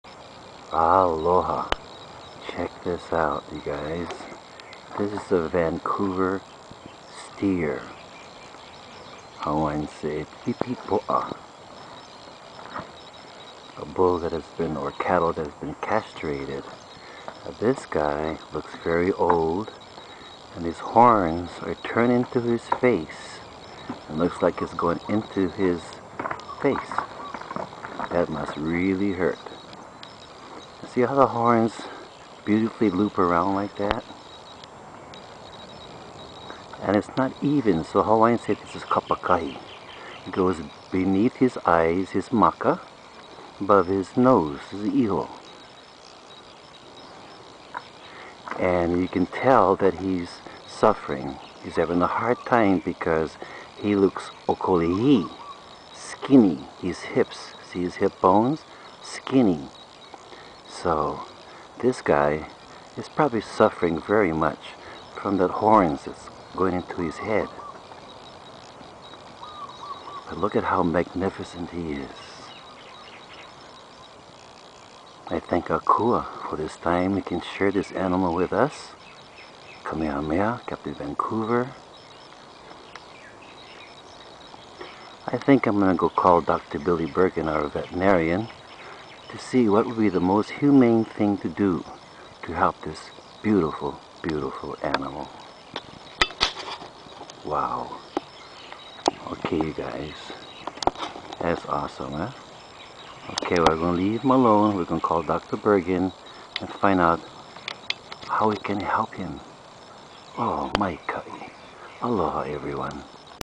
A Slack Key "Paniolo Talk Story "Thriller
Multi-talented Juni Medeiros is observing the health and well-fare of a Vancouver Steer, up close.   Son of Paniolo Hall of Fame Alfred Medeiros ( McCandless ranch), nephew to Paniolo Hall of fame uncles Gilbert (Kahuku Ranch), Johnny (Honomalino ranch), and Clarence(Magoon ranch).